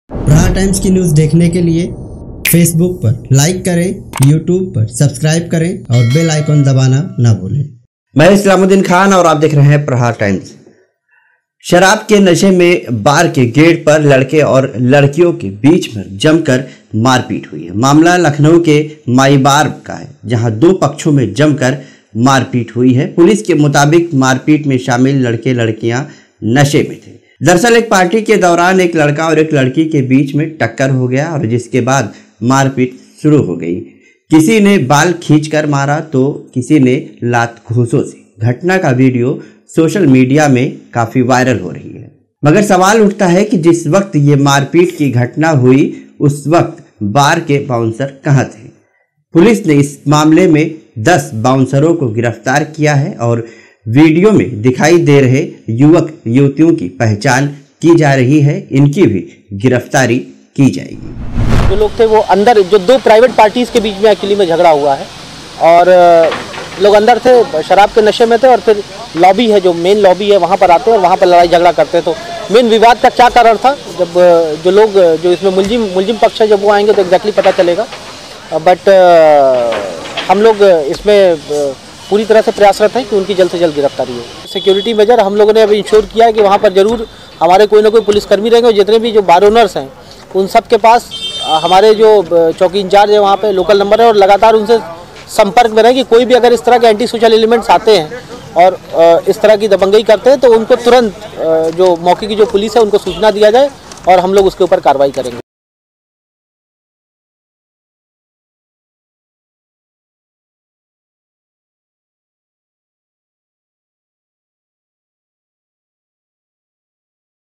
Viral Video : बार मे पार्टी के दौरान लड़के लड़कियों में मारपीट || Islamuddin Khan | Aapka Prahar Times
Viral Video : बार मे पार्टी के दौरान लड़के लड़कियों में मारपीट || Islamuddin Khan

बार मे पार्टी के दौरान लड़के और लड़कियों में जमकर मारपीट हुई जिसकी वीडियो काफी वायरल हो रही है पुलिस मामले की जांच में जुटी है